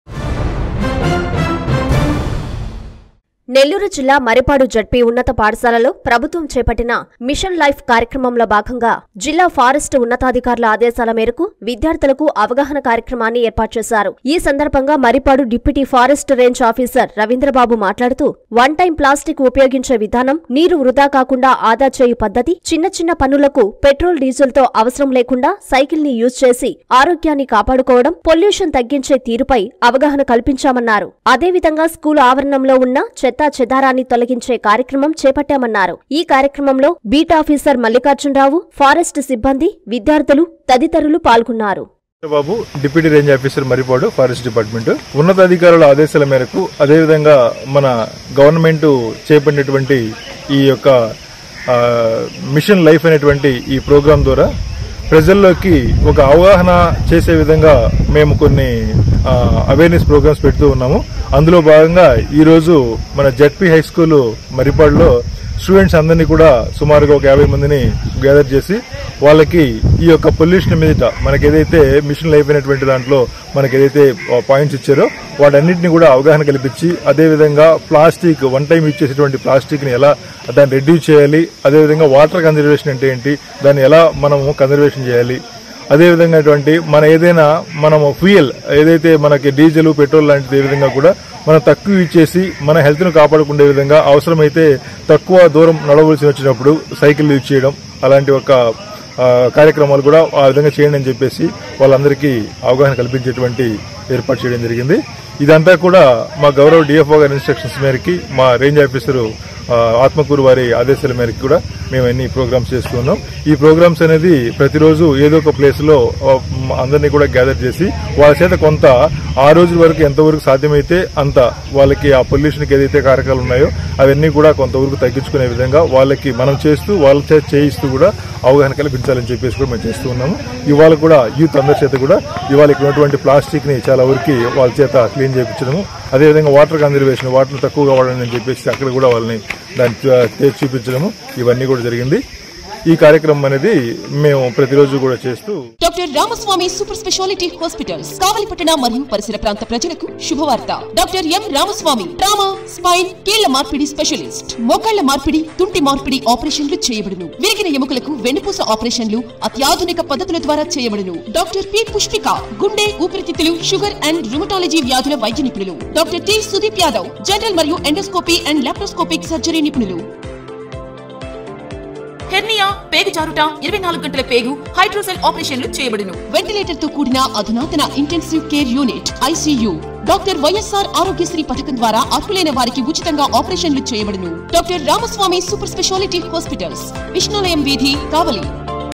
మిష‌న్‌లైఫ్‌పై అవ‌గాహ‌న‌
మ‌ర్రిపాడులో విద్యార్థుల‌కు అవ‌గాహ‌న క‌ల్పించిన డిప్యూటీ ఫారెస్ట్ రేంజ్ ఆఫీసర్ రవీంద్రబాబు